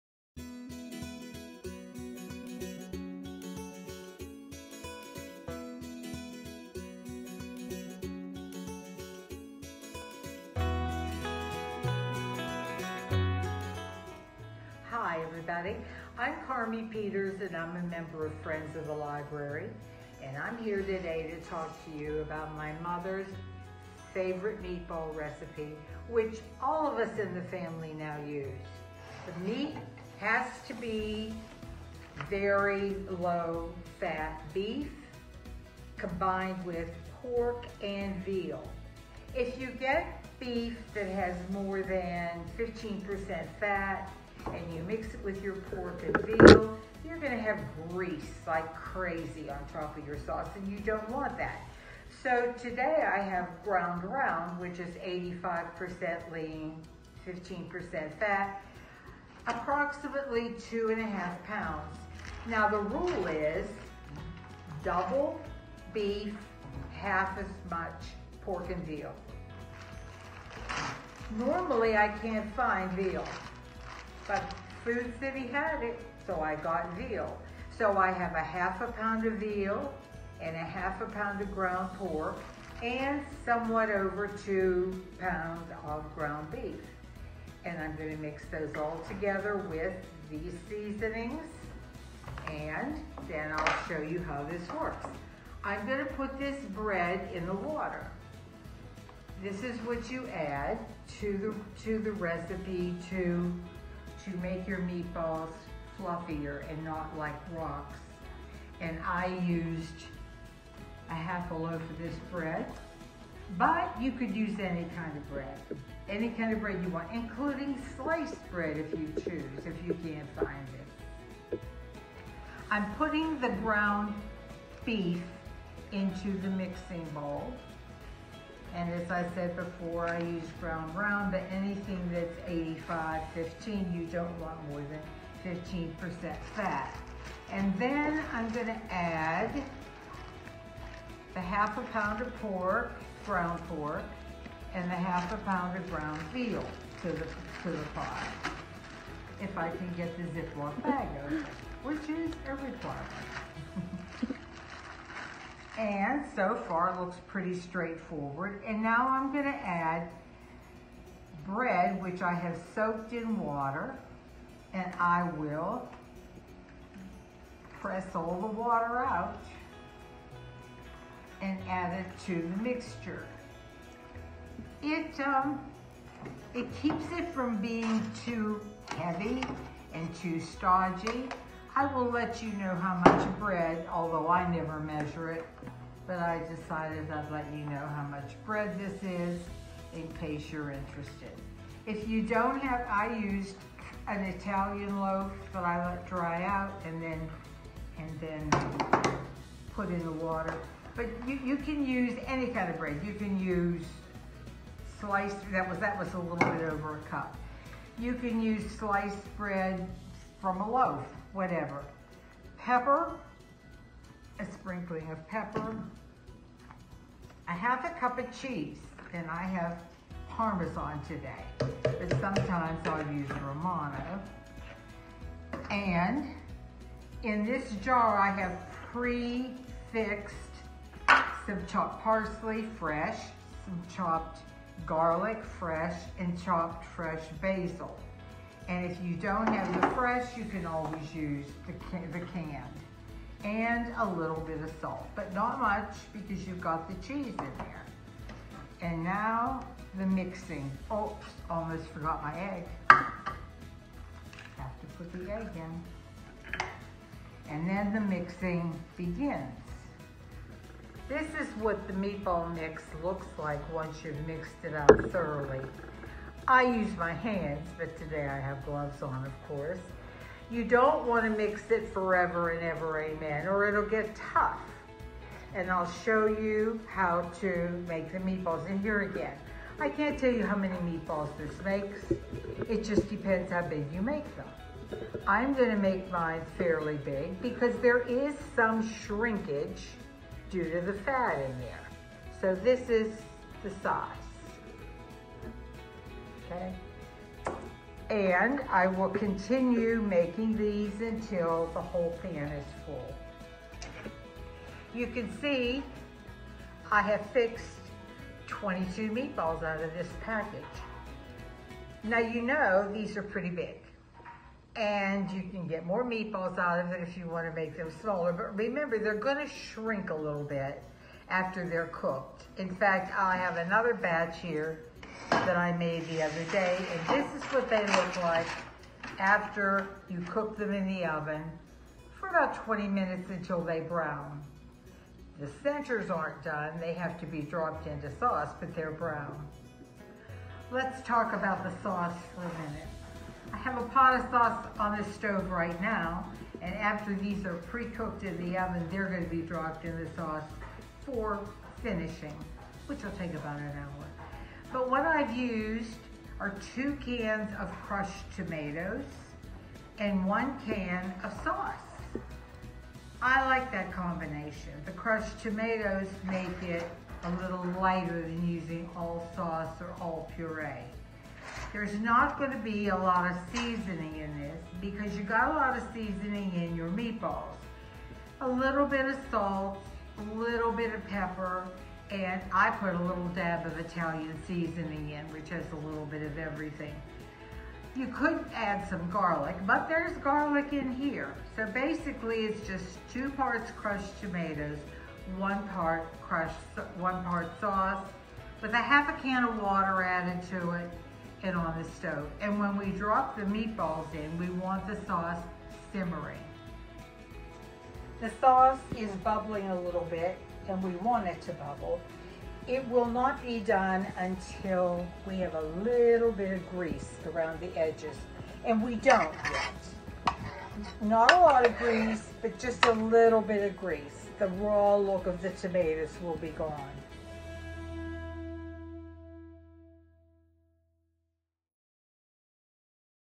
Carmie’s Cuisine
Watch as Carmie Peters, member of CML’s Friends of the Library shows us how to make her mother’s favorite meatball recipe.